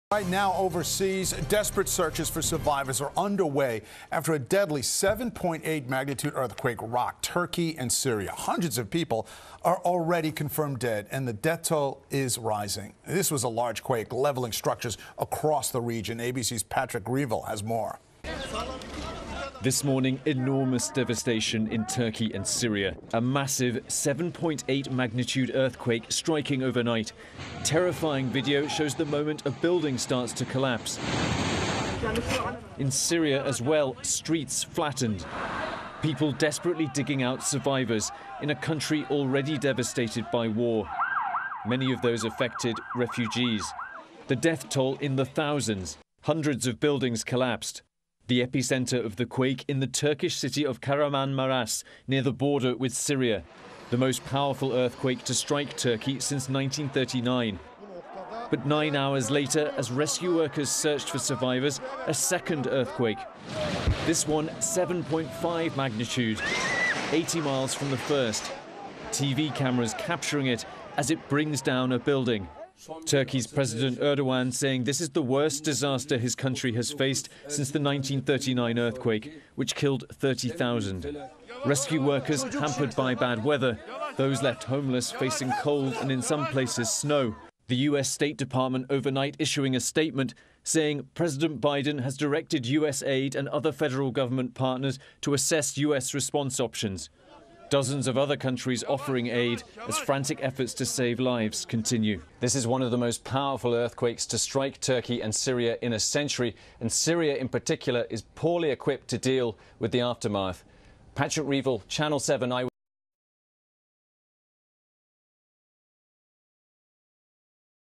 More than 2,000 people killed in earthquake in Turkey
Patrick Revell has the latest on the deadly earthquake in Turkey.

A powerful 7.8 magnitude earthquake rocked wide swaths of Turkey and Syria early Monday, toppling hundreds of buildings and killing more than 2,300 people.

Hundreds were still believed to be trapped under rubble, and the toll was expected to rise as rescue workers searched mounds of wreckage in cities and towns across the area.